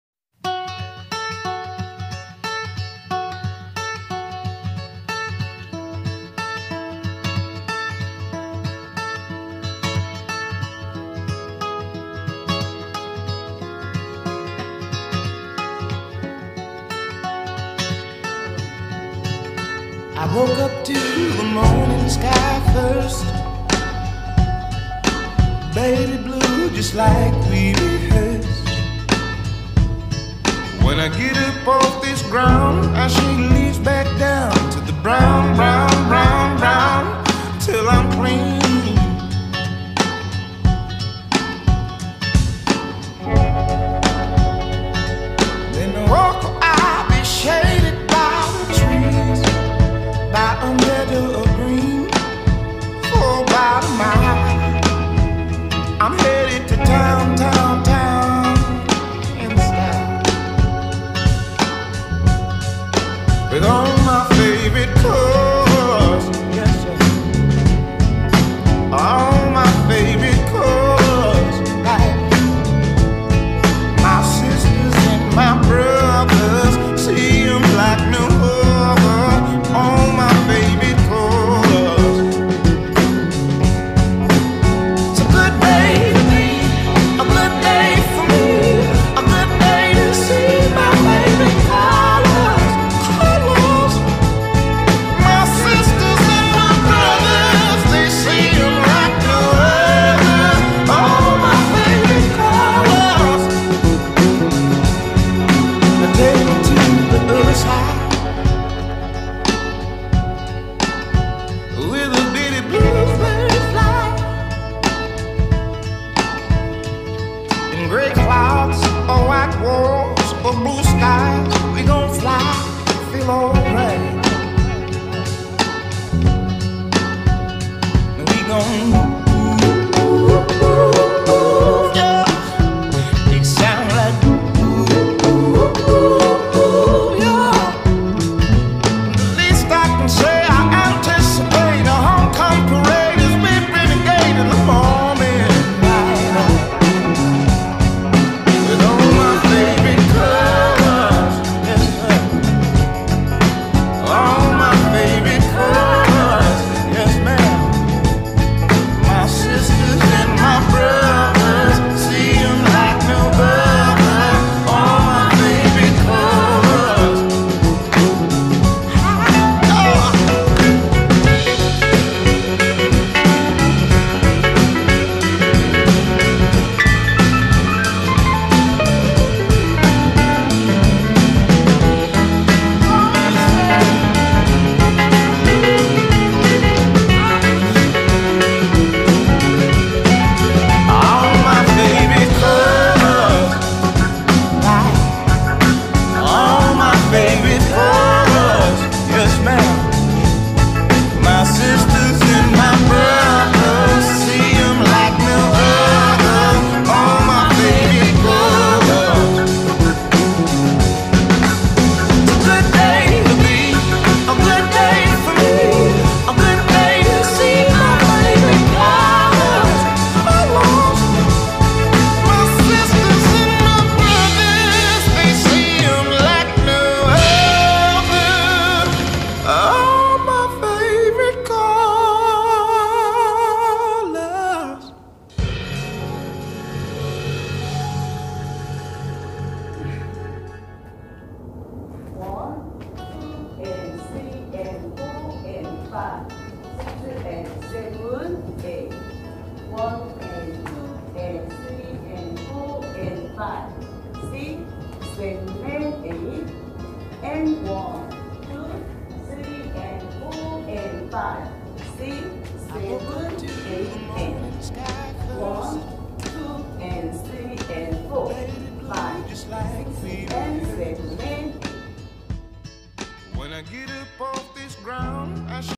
Colors Line Dance (Demo & Count)
32 C / 4 W / Intermediate
Choreographer. Gonzalez Nadja van den Eeden